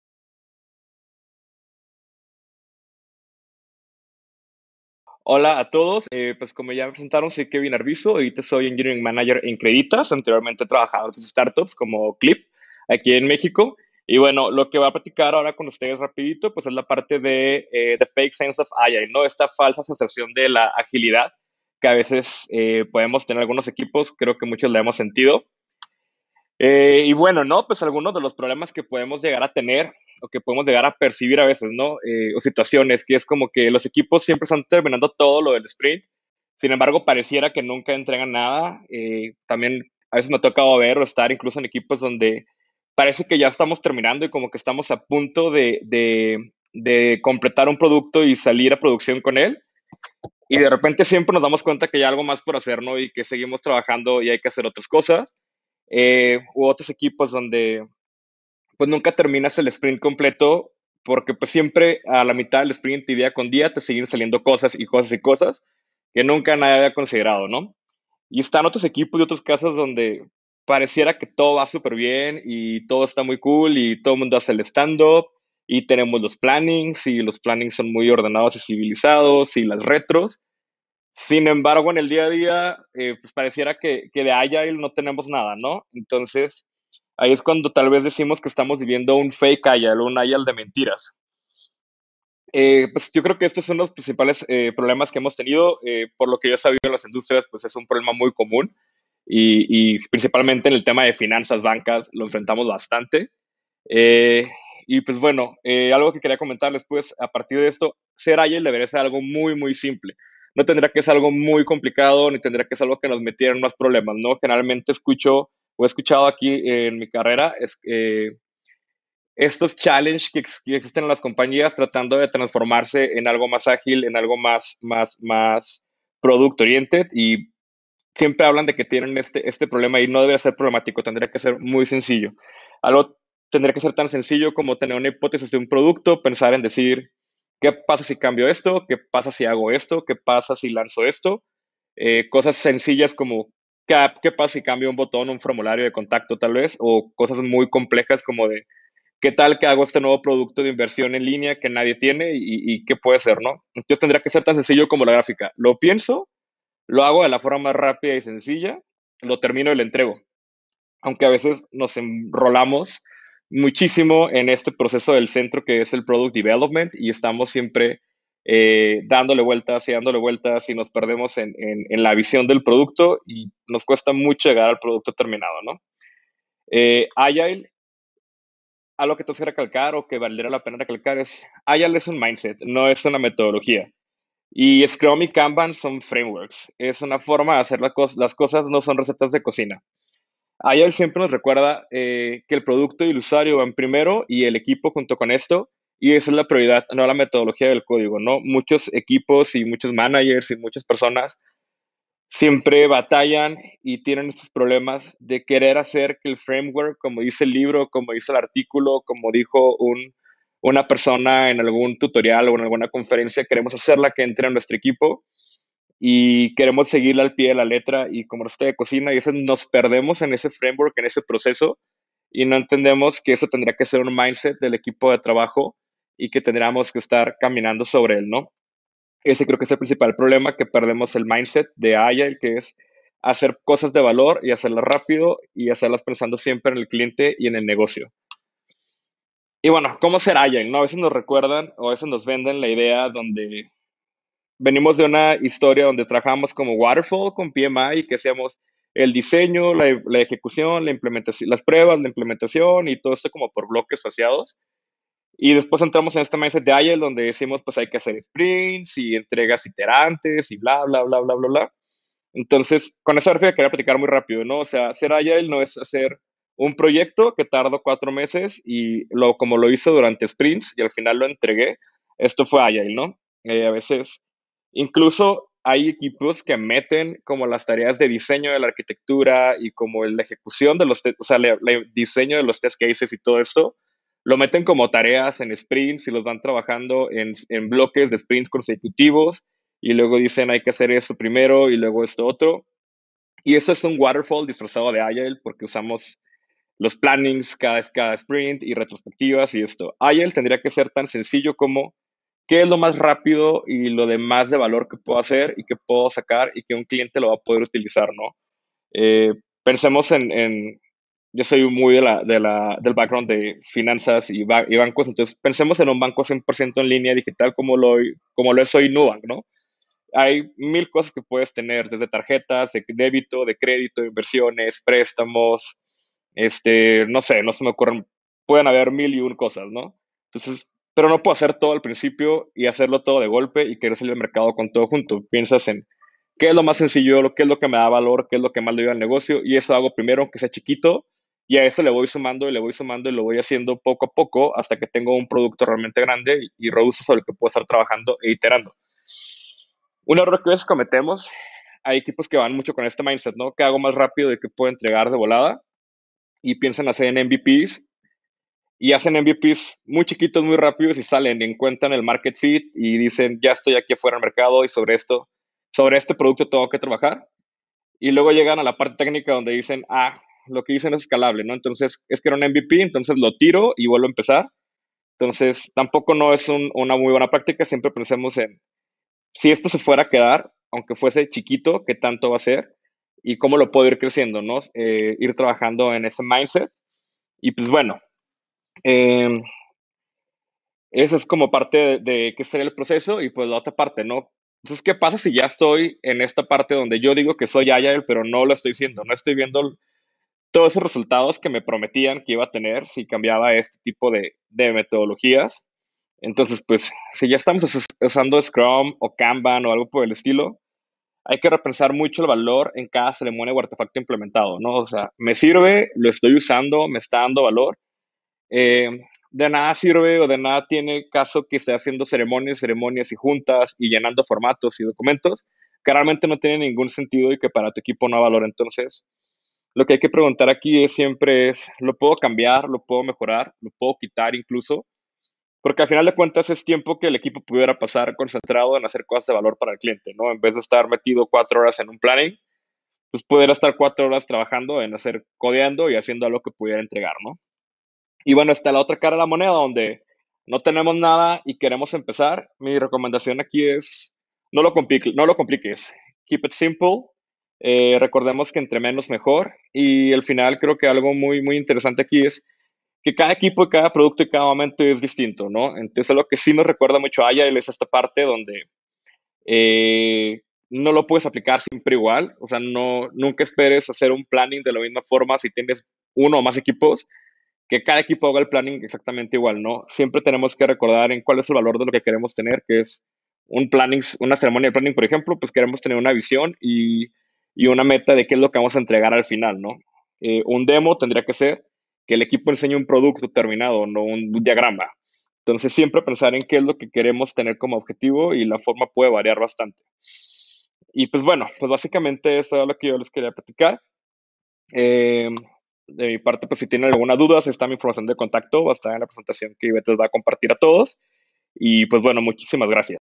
SGVirtual 20.04 La falsa noción de Agile
¿Realizas diario tus juntas stand-up y juntas de planeación pero sin demo de productos? Tal vez tu equipo se encuentra atorado en la falsa noción de que es Agile. Hablemos de Agile como un estado mental y no como un proceso, y veamos cómo puedes ayudar a tu equipo a tener resultados maravillosos.

Sesión presentada por Kevin Arvizu en SG Virtual 20.04.